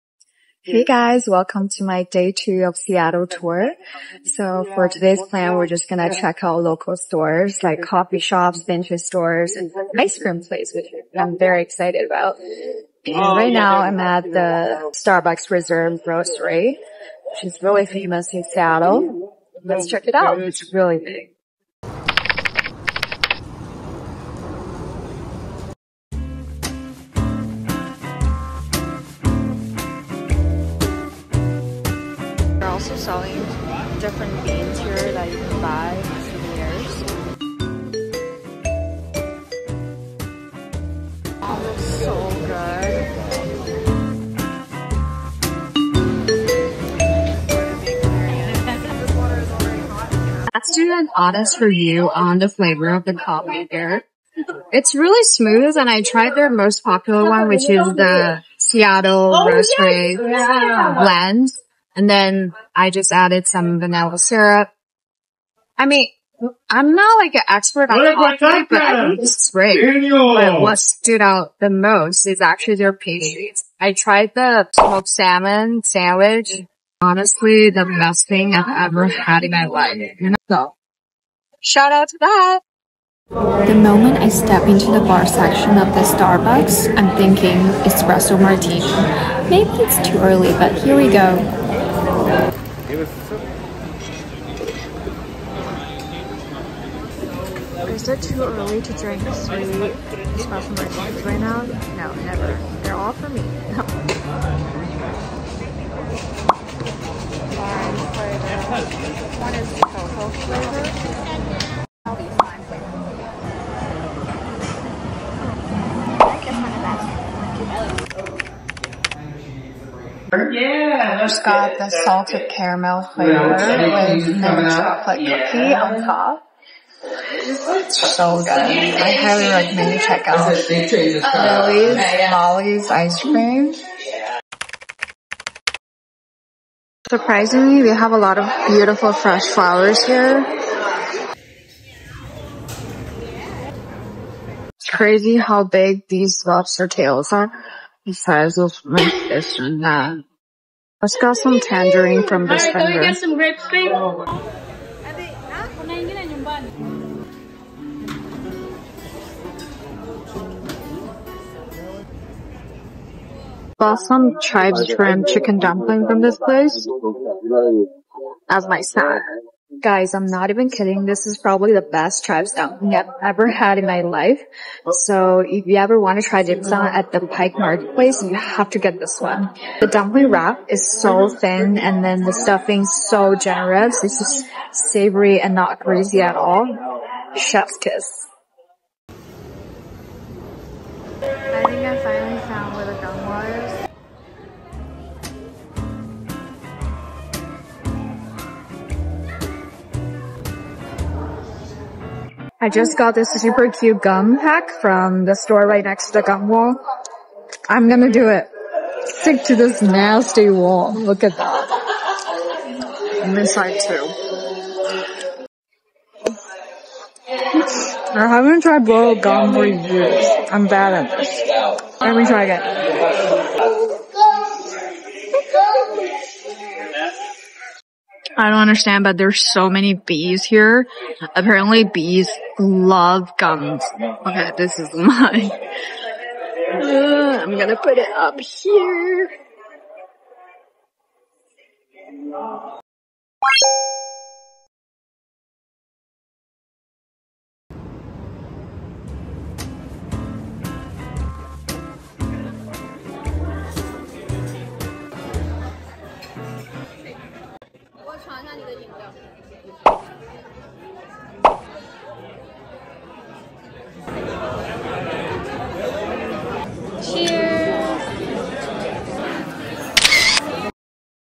Vlog| Seattle Day 2| Drinking 3 espresso martini in the morning to start my day 😂 | Starbucks love
** 所有视频会在YouTube首发。如果喜欢我的视频，
小红书：Amy说一说
Instagram账号：ciaoamychen